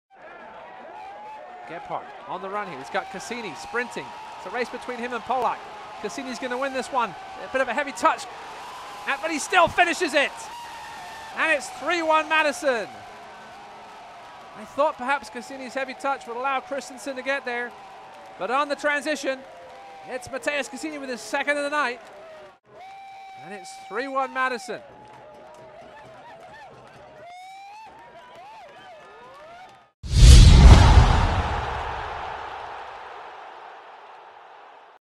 Matheus Henrique Cassini de Paula with a Goal vs. Greenville Triumph SC
Matheus Henrique Cassini de Paula (Forward Madison FC) with a Goal vs. Greenville Triumph SC, 06/11/2022